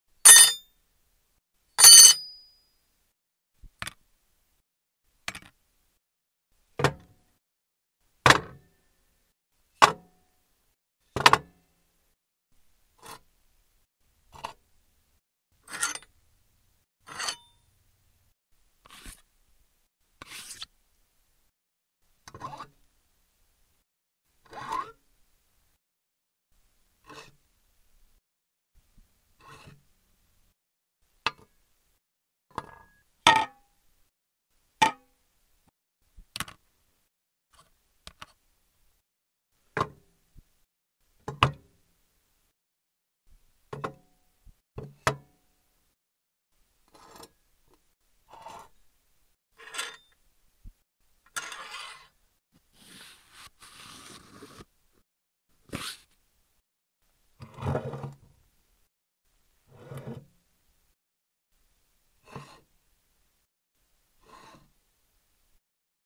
Objects on Metal - Physics Sound Effects - Slide, Drop, Place, Pick Up
Includes 4 movements and 5 materials used. Movements feature pick up, place, slide and drop. Materials include glass, metal, stone, wood and plastic.

Add dynamic sounding audio to your project with our Objects on Metal sound pack. Create a realistic environment with sound, does your character slide a stone on a metal surface? We have sound for that!